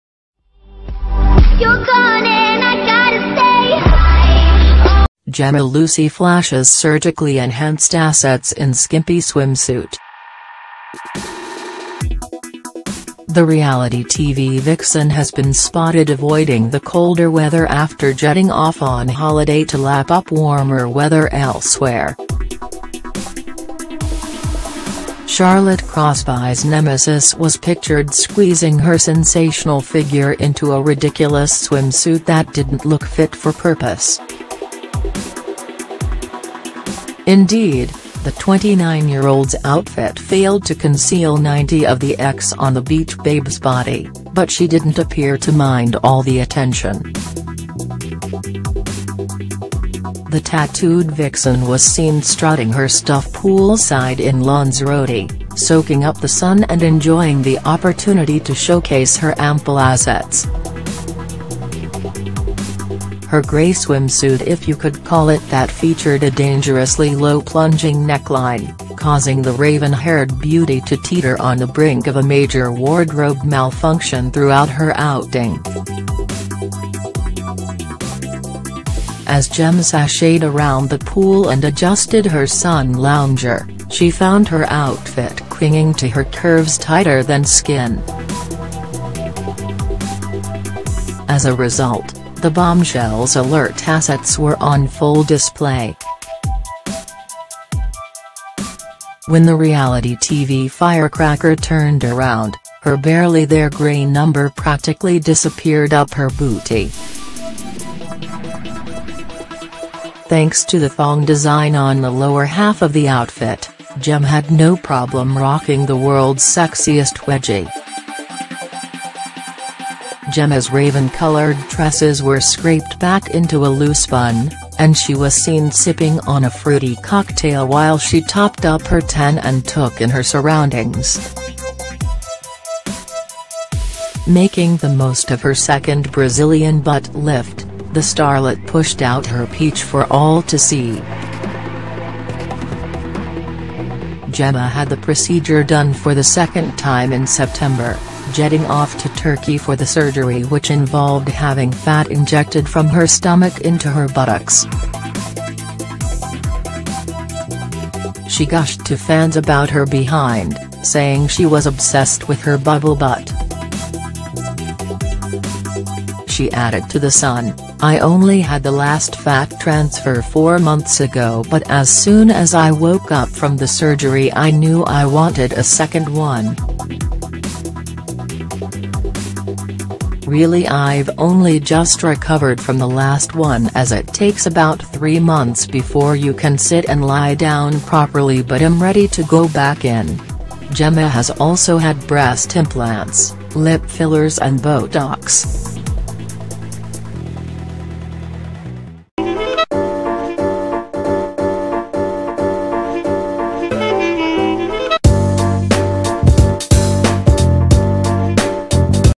Jemma Lucy flashes surgically-enhanced assets in skimpy swimsuit
CELEBRITY Big Brother 2017 may be over, but Jemma Lucy's flesh-flashing has only just begun.